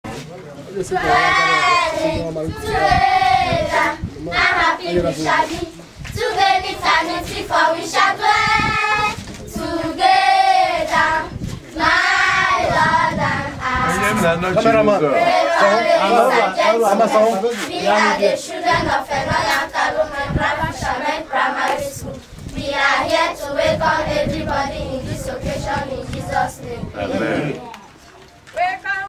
Enohia Migrant Fisherman Primary School
July 9th, 2009 Wings of the Dawn official visit to Enohia Migrant Fisherman Primary School

Enohia Migrant Fisherman Primary School is in the village of Enohia Nkalu, Afikpo, Nigeria, the village is the smallest in the local government area situated in the extreme East of the Niger delta State about 10 kilometers from the majority of other villages (so more or less isolated), separated from Cross River State by cross river. The population at the last census was 5654, of mainly younger age groups because of adult mortality for obvious reasons. more than 90% of the adult working group are fishermen, who travel to River Niger or some of its tributaries in the Rivers State to fish. They come back to the village only during the heavy rains (July to September).

Currently the school is being housed in two uncompleted personal (residential) buildings that the owners have kindly allowed the community to use temporarily for the school. Wings of the Dawn in collaboration with high school students from Trinity Valley School Fort Worth, Texas is currently raising needed fund to help complete a six block classroom building for the community school project.